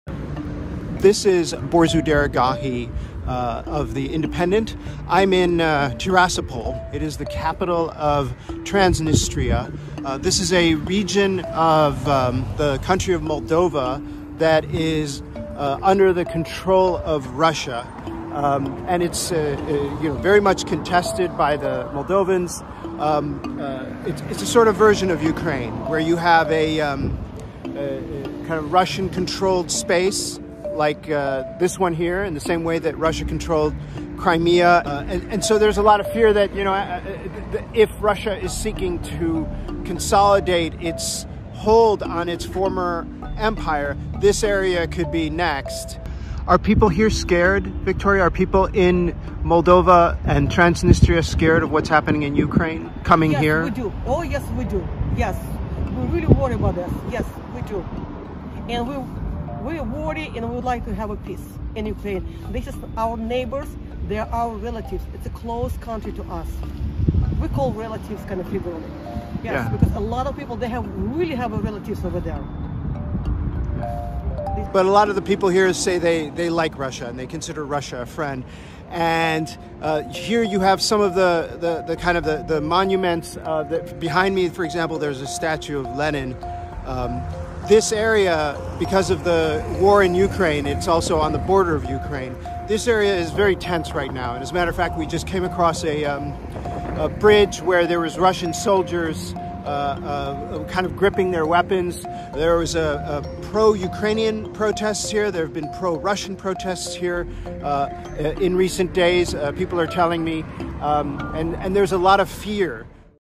Moldova: Inside Transnistria, the pro-Russia enclave on Ukraine’s border
Along the bridge, the Russian soldier gripped his assault rifle tightly and aimed when a passing vehicle did not stop fast enough to his liking. Since Russia’s war against Ukraine, next door there have been rare outbreaks of political unrest in Transnistria, the Kremlin-controlled breakaway region of Moldova

One woman  said pro-Ukrainian “saboteurs” had attempted to stage an anti-Russian protest that was quickly suppressed, followed by a larger pro-Russian demonstration. Then came a public plea from nervous authorities: no more political displays.

Across the demarcation line in Chisinau and other Moldovan towns, fears are also mounting as the struggling country, among the poorest in Europe, welcomes and shelters tens of thousands of Ukrainian refugees flooding in from the war next door.